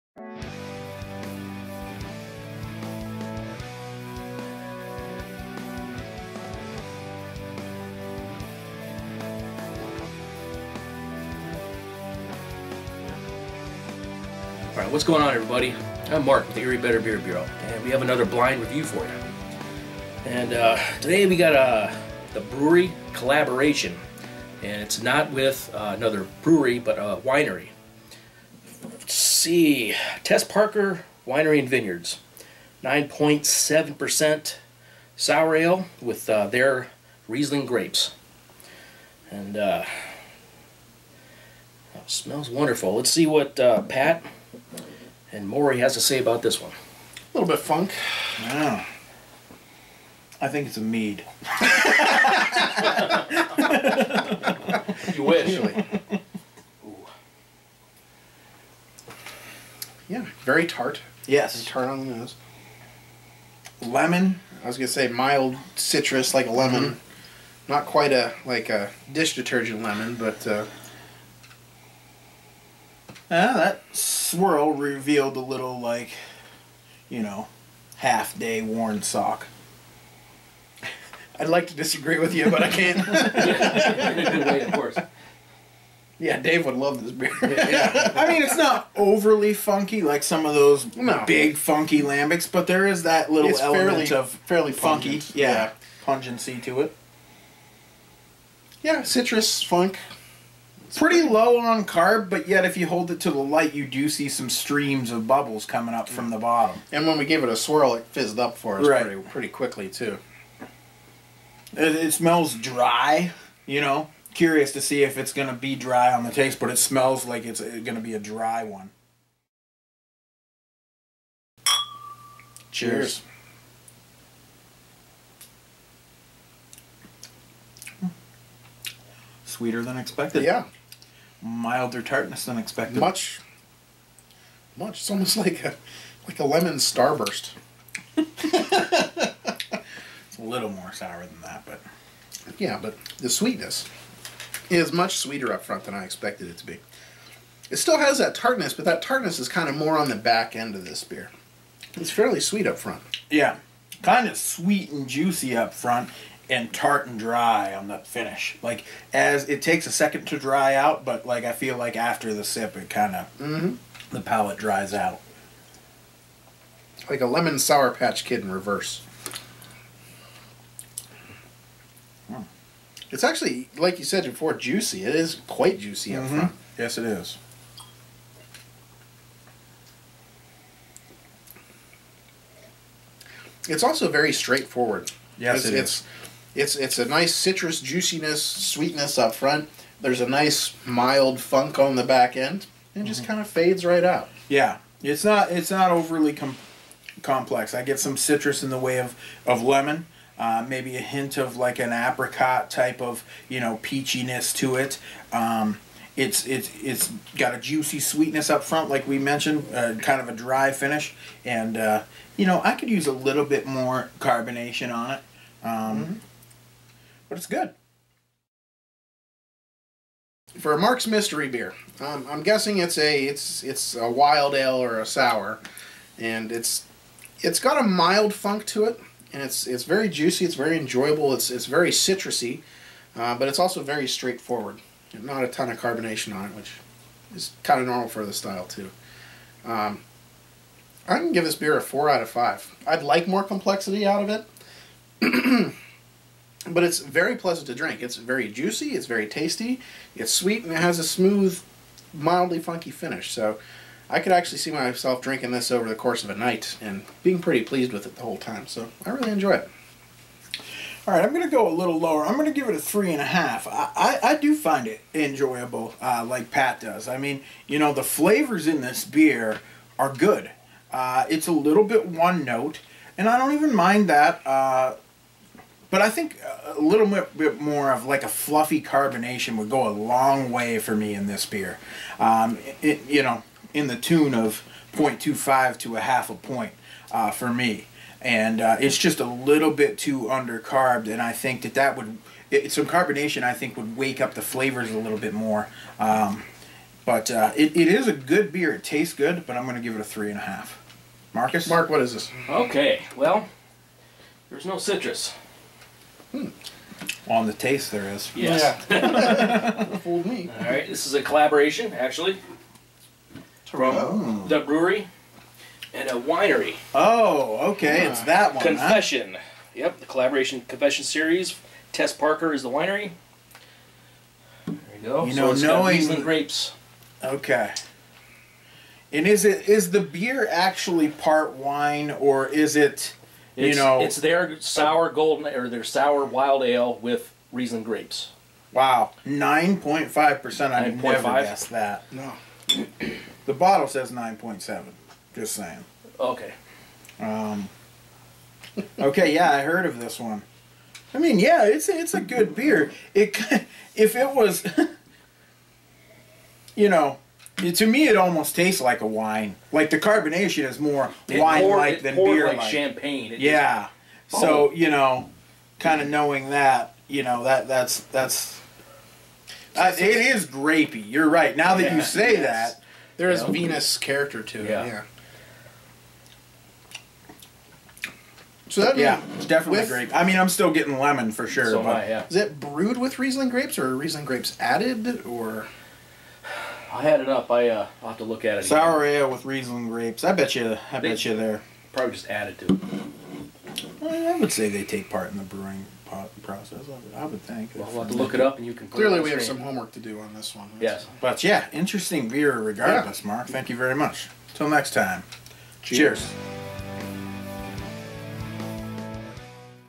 EBBB: The Bruery Confession (BLIND) - Review #363
Description: Confession is a true collaborative effort between the wine and beer world. A variation of our sour blonde ale, brewed in part by Fess Parker Winery, was blended and fermented with juice pressed from Parker's Riesling grapes, harvested in part by The Bruery team. The result is remarkable. Not quite beer, not quite wine. Fragrant fruit flavors warm the nose, infiltrated by a slight funk from the wild yeast. The first sip is reminiscent of a dry, white wine, but everything changes upon further examination as the flavors marinate and reveal themselves with each gulp.